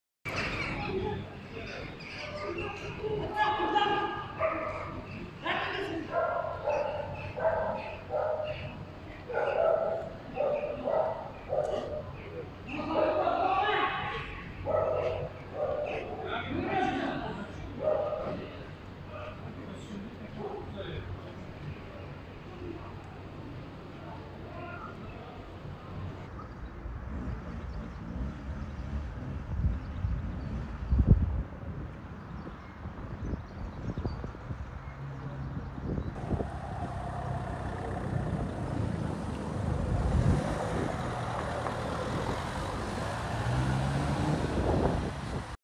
Cezaevine götürüldüğü esnada adliyeden kaçmaya çalıştı!
Sakarya’da hakkında tutuklama kararı verildikten sonra adliyeden cezaevine götürülmek isteyen şahıs kaçmaya çalıştı. ‘Dur’ ihtarına uymayarak kaçmaya çalışan şahıs için polis havaya uyarı ateşi açtı. Şahsın yakalanarak etkisiz hale getirildiği anlar ise cep telefonu kamerasıyla görüntülendi.